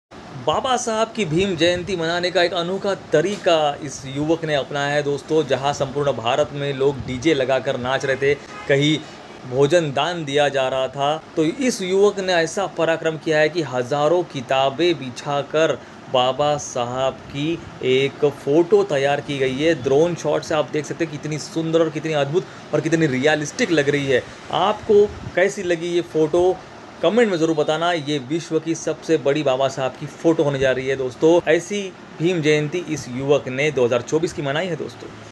किताबो से बनाई बाबा साहेब की दुनिया की सबसे बडी फोटो 💙 Bhim Jayanti Special 💙
Follow Mithilesh Ganar On
YOUTUBE... MG Vlogs.

WHATS APP CHANNEL - MG Vlogs..